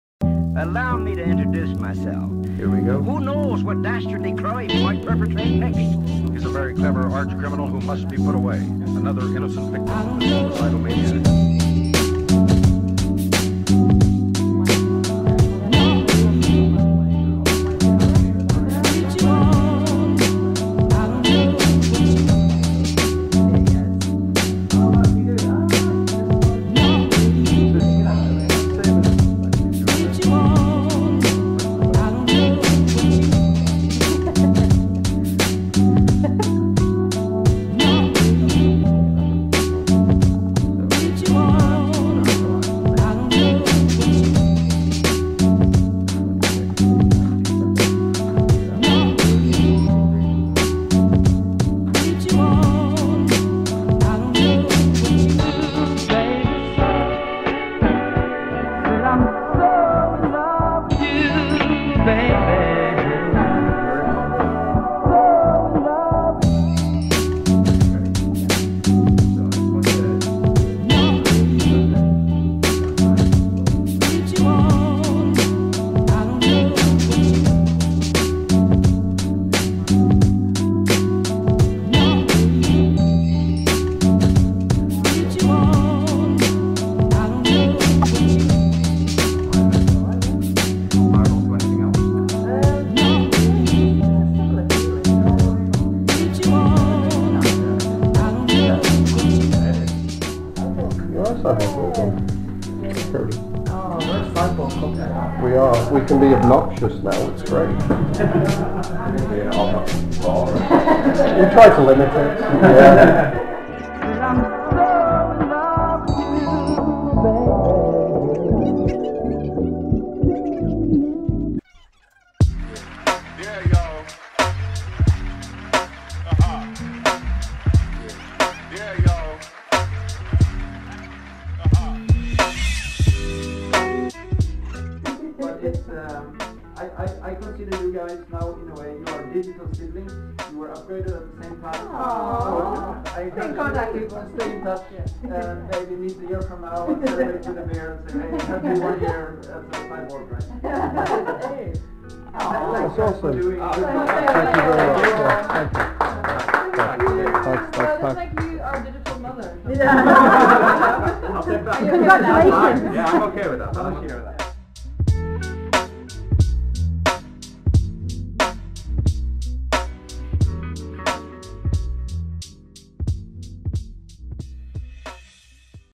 Life as a Cyborg - Day 0 - Implantation
Warning: Scenes of Piercing but No Blood.

On the 22nd of April 2017 my partner and I became Cyborgs when we were implanted with a Dangerous Things xNT NFC Tag.
This NFC implant sits just under the skin and allows me to access certain doors and interact with people's phones.

Tech Spec:
xNT tag – 13.56MHz ISO14443A & NFC Type 2 NTAG216 chip
2x12mm cylindrical sterile biocompatible implant package
ISO14443A – compatible with all ISO14443A RFID systems
Fully NFC Type 2 compliant – compatible with all NFC devices

More info to come in the coming weeks.